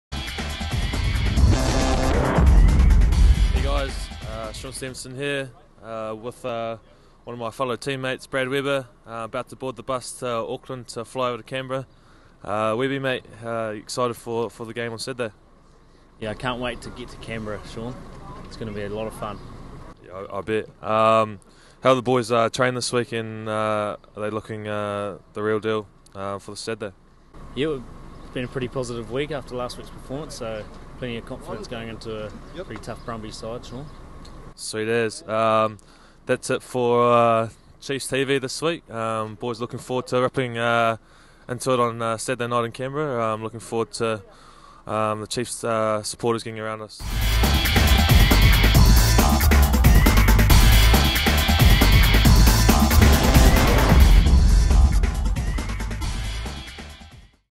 Chiefs TV - Shaun Stevenson and Brad Weber
Shaun Stevenson and Brad Weber prepare to hit the Brumbies in Canberra